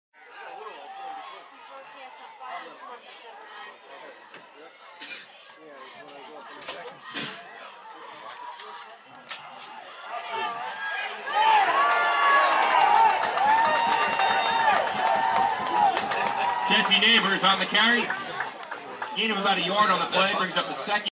Big hit by Max Pfrang
Rumson junior linebacker Max Pfrang lays a big hit on a Delaware Valley ball carrier during the Bulldogs' 21-0 win for their second straight NJSIAA Central Jersey Group II championship.